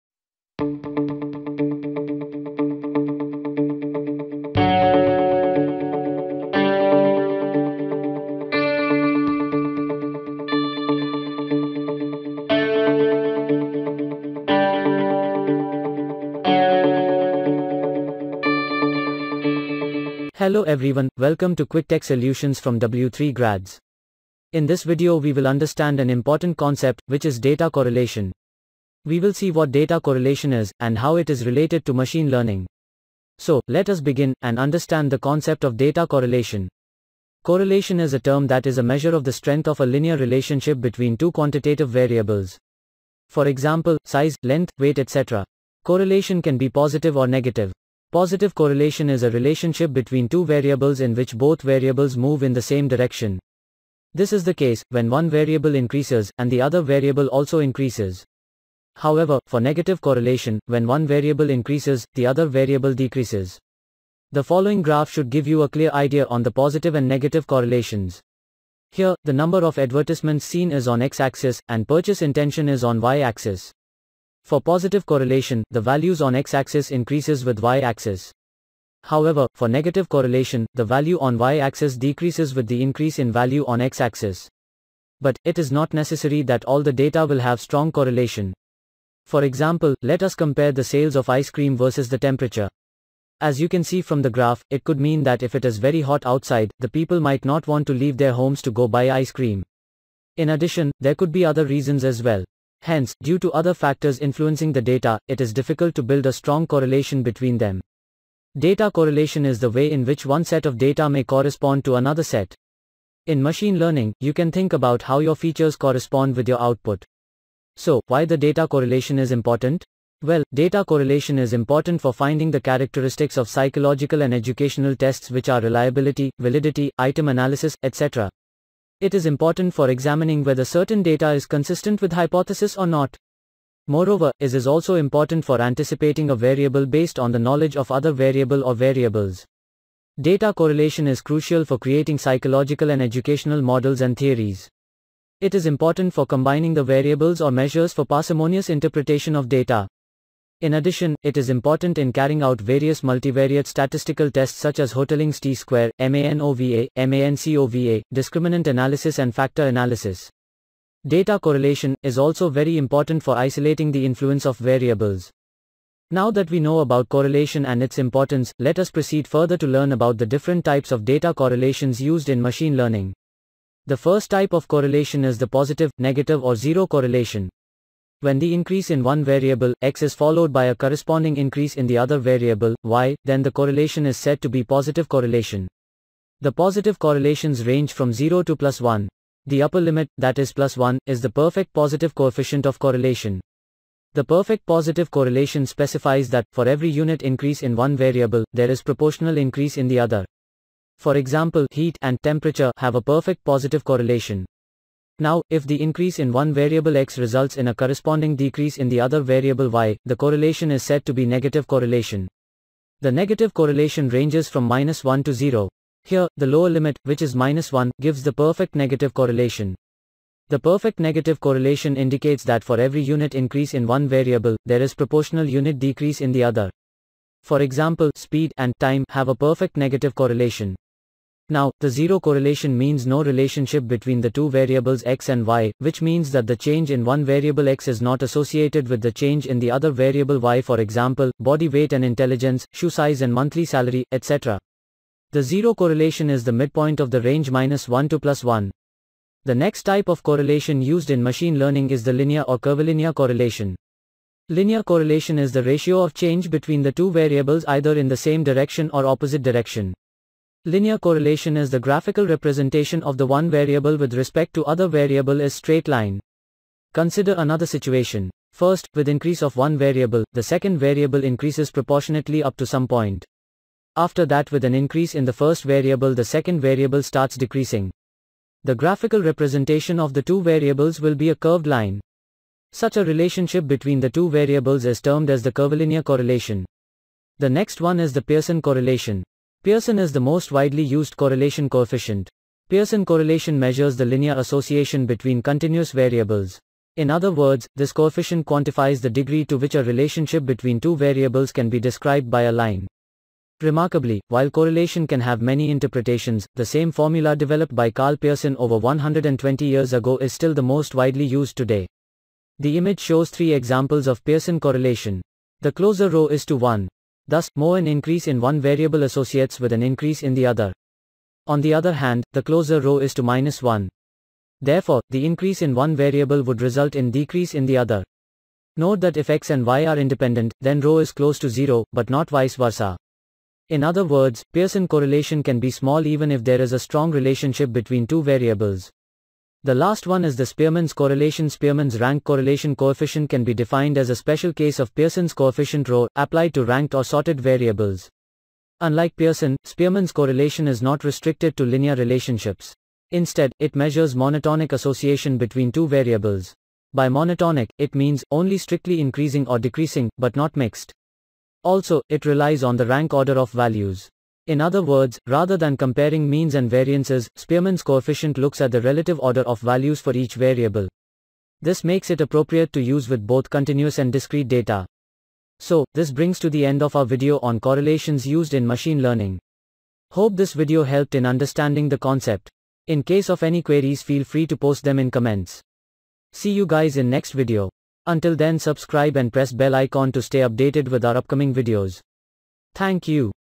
What is Data Correlation in Machine Learning | Types of Data Correlation | Pearson and Spearman
Data Correlation is the most important aspect in Machine Learning, as this forms the decision made based on data feed. And when looking at data correlation in machine learning there different ways where the data can be interpreted. This explains the different types of data correlation available in machine learning.

Location: Ground Floor, 118/362, A Block,
Kaushalpuri - Gumti No. 5, (Near Gurunanak Dharamsala)
Kanpur 208012 (U.P.)